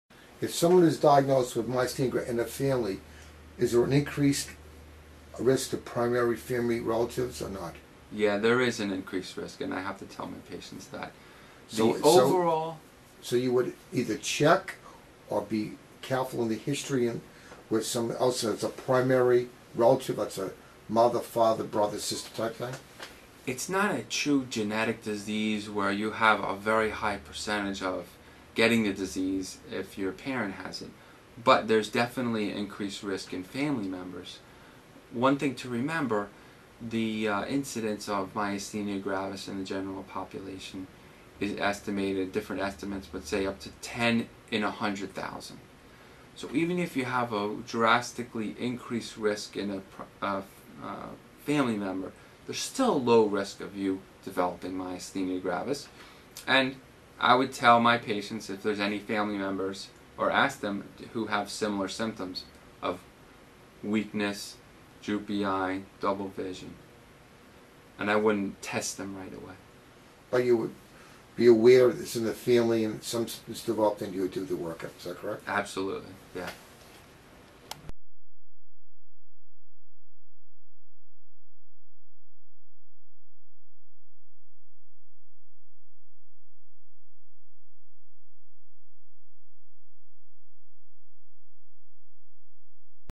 Myasthenia Gravis risk to other family members
Cary D. Buckner, M.D.
New York Methodist Hospital, Vice Chairman, Department of  Neurosciences |
Myasthenia Gravis Alliance - Medical Advisor | Produced by: DrMDK.com